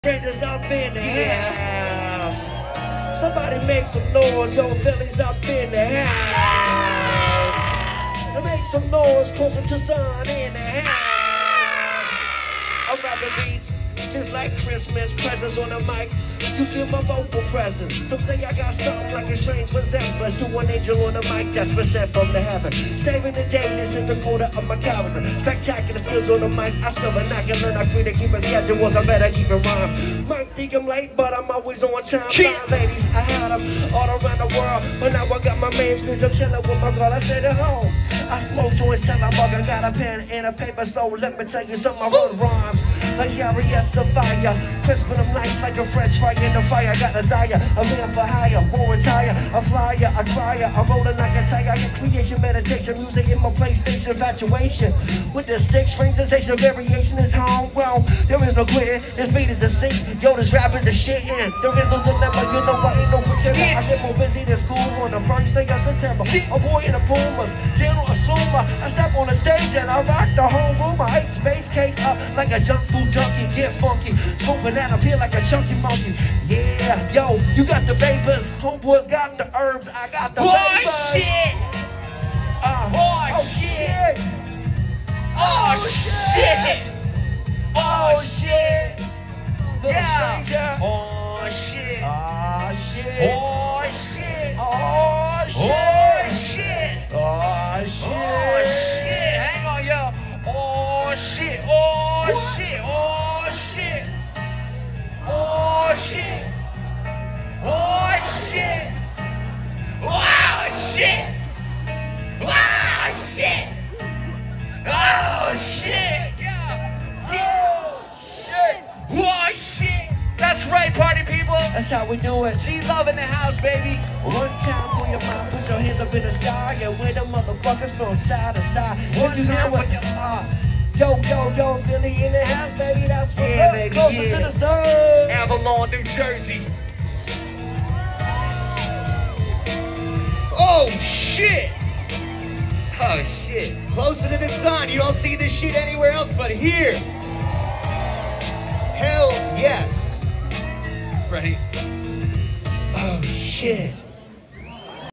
& @phillyglove at @ctts 2023!!
Another @LittleStranger and @phillyglove set, all the way live from the 215!!! Nipple crew 2023 CTTS!!